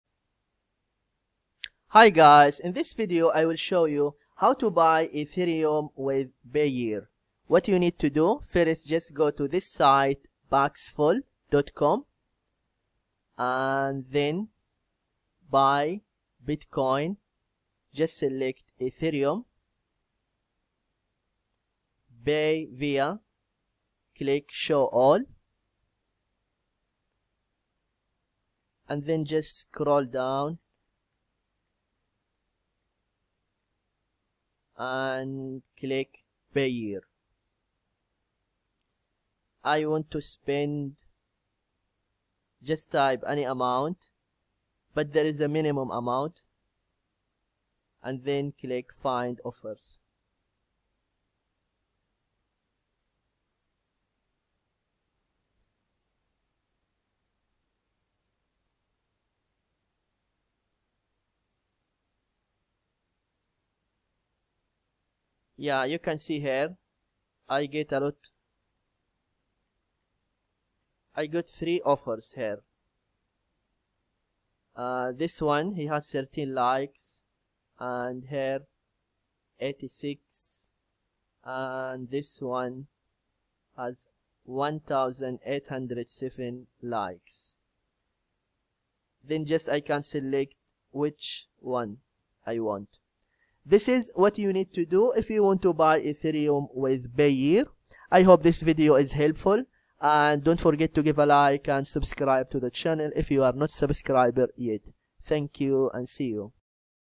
How To Buy Etherum With Payeer 2022
Stay secure online and hide from your ISP with IVACY VPN!

In this video, I will show you How To Buy Etherum With Payeer 2022